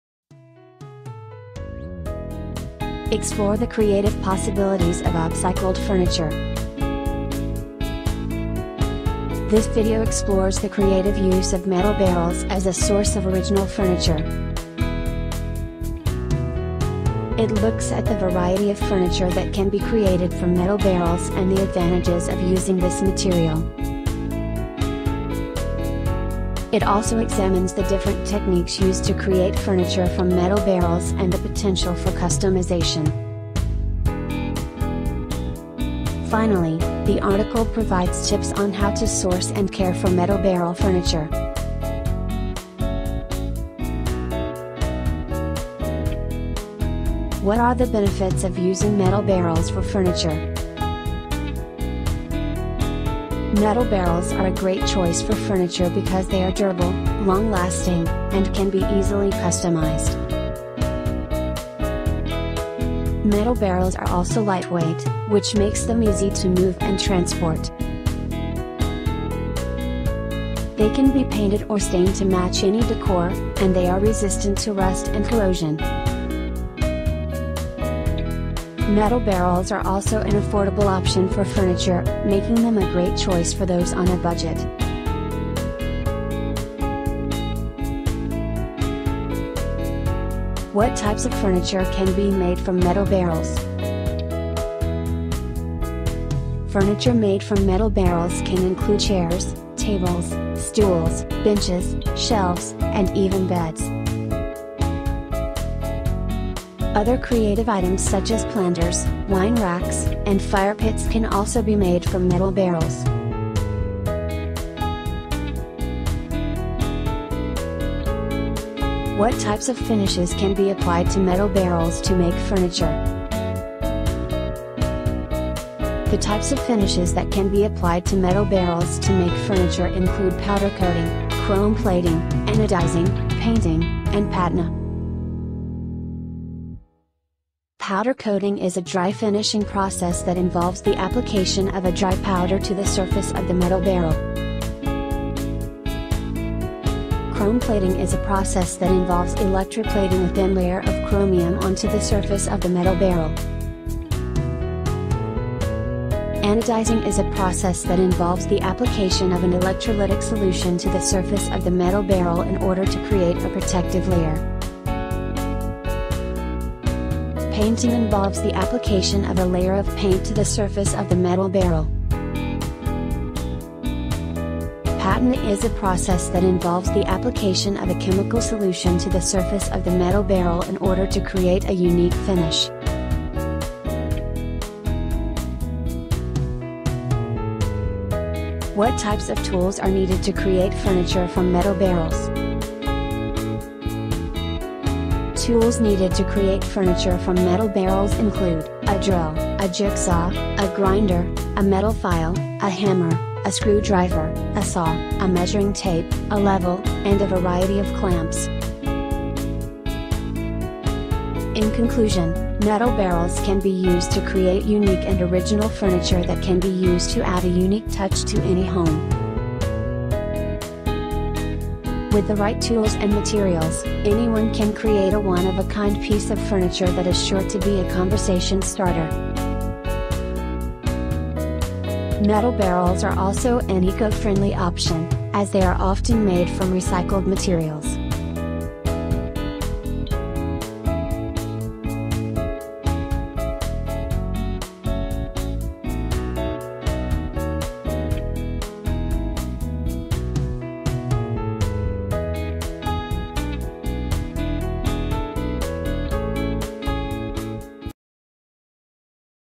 Original Furniture From Metal Barrels | Recycled Metal Drum Projects
Explore the Creative Possibilities of Upcycled Furniture

This video explores the creative use of metal barrels as a source of original furniture.
 It looks at the variety of furniture that can be created from metal barrels and the advantages of using this material.
 It also examines the different techniques used to create furniture from metal barrels and the potential for customization.
 Finally, the video provides tips on how to source and care for metal barrel furniture.
Recycled Metal Drum Projects.
Original furniture ideas from metal barrels.
Furniture for the garden and cottages from used metal barrels.
Interesting ideas for inspiration.
  What can be made from metal barrels - crafts and homemade products for summer cottages and country houses. New life for an old barrel.
Furniture that you can make yourself.
Beautiful barrel furniture.
In conclusion, metal barrels can be used to create unique and original furniture that can be used to add a unique touch to any home.
 With the right tools and materials, anyone can create a one-of-a-kind piece of furniture that is sure to be a conversation starter.
 Metal barrels are also an eco-friendly option, as they are often made from recycled materials.